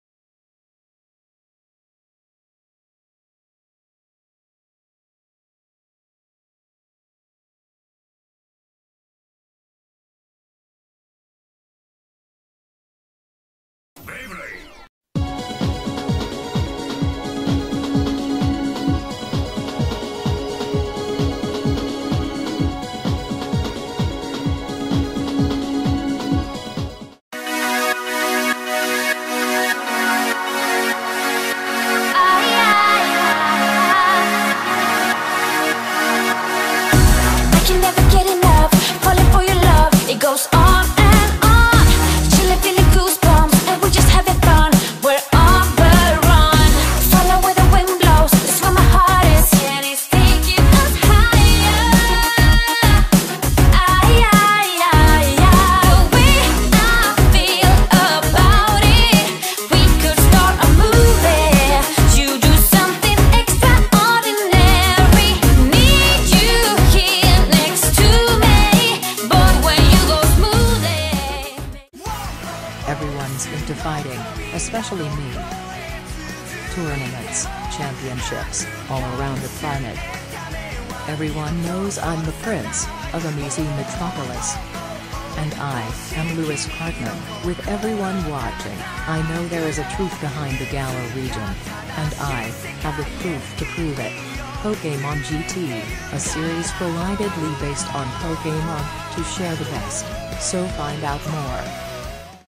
Pokemon GT Episode Showcase: Sundays are Blank (Part One)
[Quick Note]: New Season 2020 Starters have Appears! See the puzzle pieces in the Video!!

Episode 7 will be a tournament episode! 2 vs 2! Blue Team vs Red Team!!
Monk-rab vs Raccoon! Airs April 5, 2020!